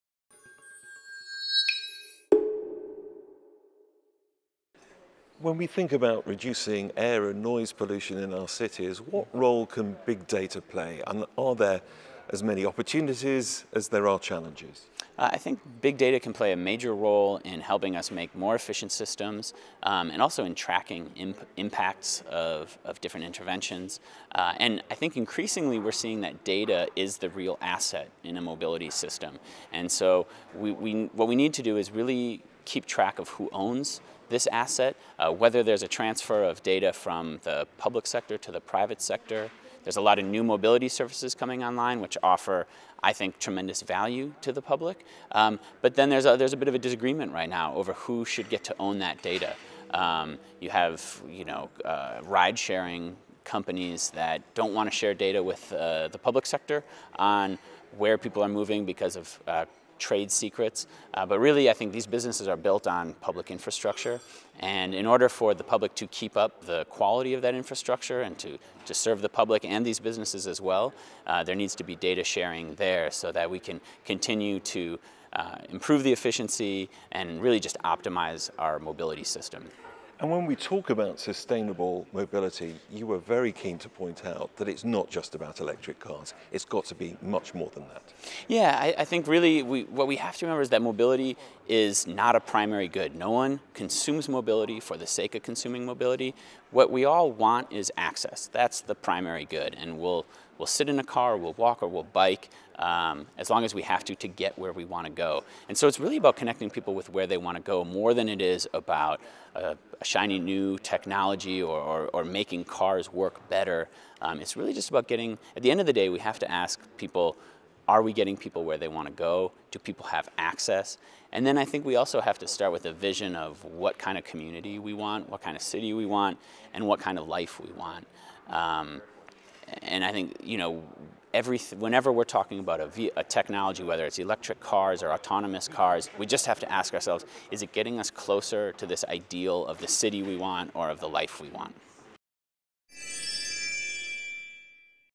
Colin Hughes (ITPD)
Interview with Colin Hughes, Institute for Transportation Policy and Development, at the International Transport Forum’s 2016 Summit on “Green and Inclusive Transport” in Leipzig, Germany on 20 May 2016 in Leipzig, Germany.